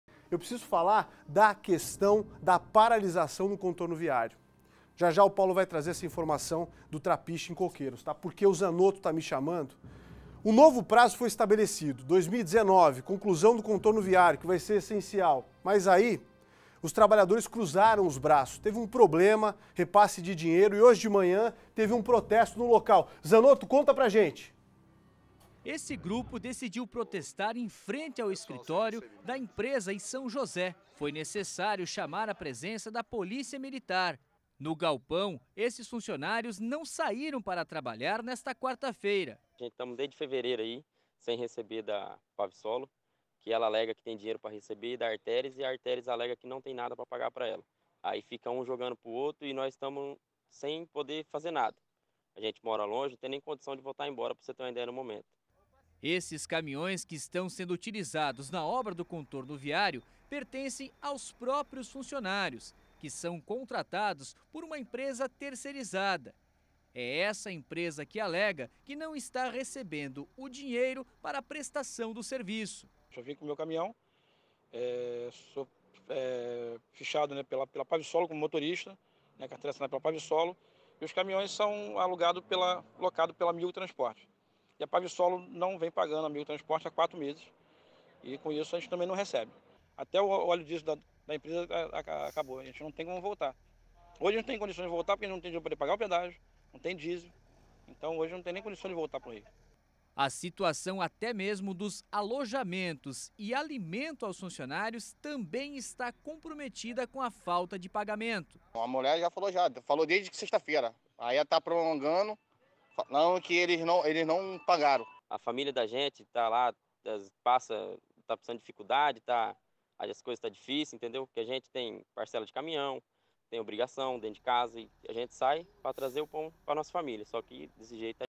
Funcionários paralizam obras do contorno viário por falta de repasses
O novo prazo para a conclusão das obras do contorno viário da Grande Florianópolis é 2019. Mas os problemas só continuam e aumentam. Hoje pela manhã teve protesto e fornecedores pararam as atividades por falta de pagamento por parte da concessionária que administra a rodovia.